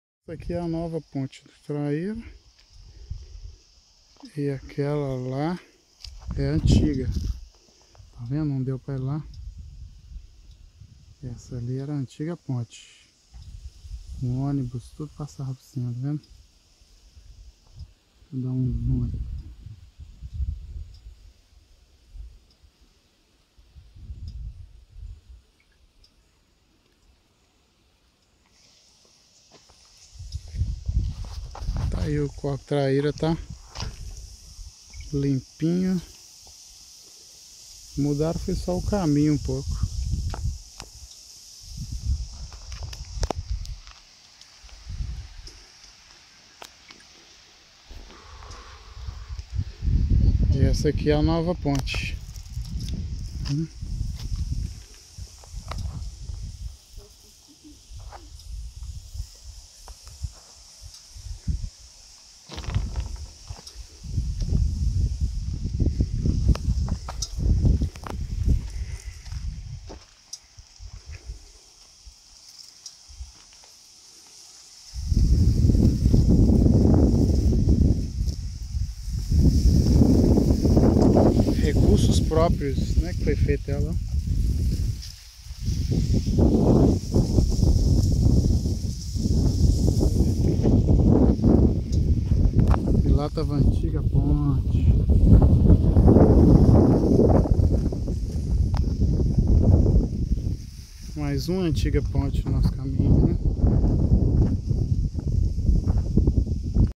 Nova Ponte Rio Traíra em córrego do Prata Tumiritinga ou Governador Valadares MG
mostramos para vocês a nova e a antiga ponte no Rio Traíra.